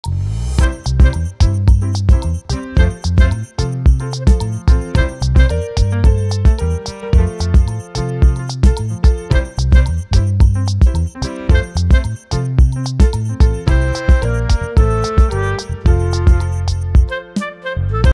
Quiet Book - Detská kniha na učenie
Vynikajúca vzdelávacia hračka založená na metóde Montessori bola vyvinutá pre batoľatá a umožňuje im rozvíjať učebné zručnosti, zlepšovať pamäť, praktické schopnosti a vizuálne vnímanie. Je vhodná pre deti od dvoch rokov. Objednajte si našu Quit Book a postarajte sa o hodiny pokojnej zábavy pre vaše dieťa!